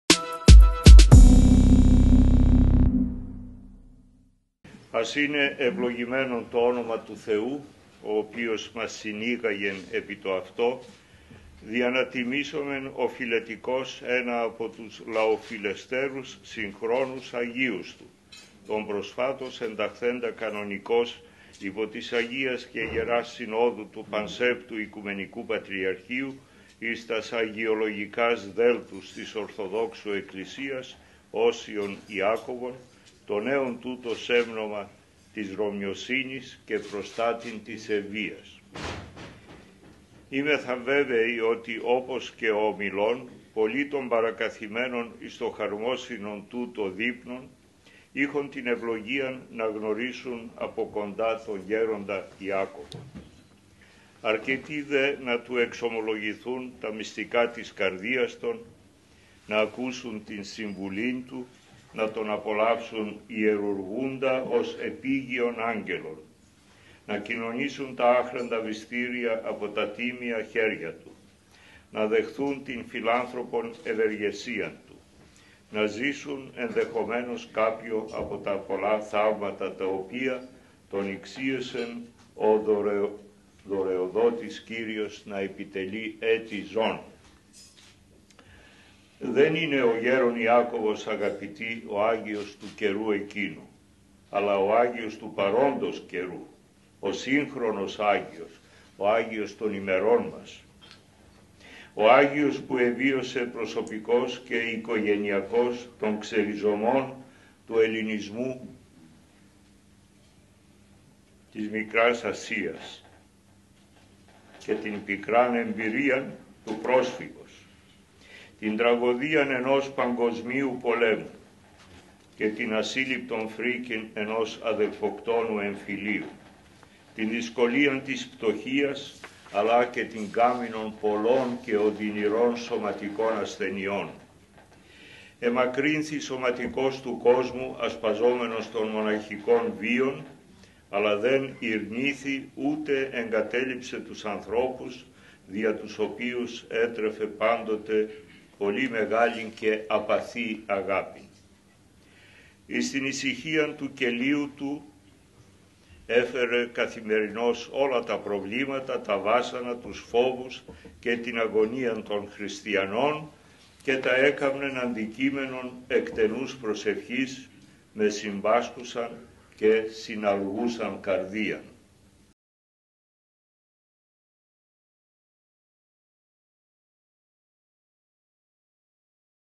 Ομιλία του Οικουμενικού Πατριάρχη Βαρθολομαίου για την Αγιοκατάταξη του Οσίου Ιακώβου του Εν Ευβοία
Ομιλία του Οικουμενικού Πατριάρχη Βαρθολομαίου για την Αγιοκατάταξη του Οσίου Ιακώβου του Εν Ευβοία. πηγή:Star Κεντρικής Ελλάδος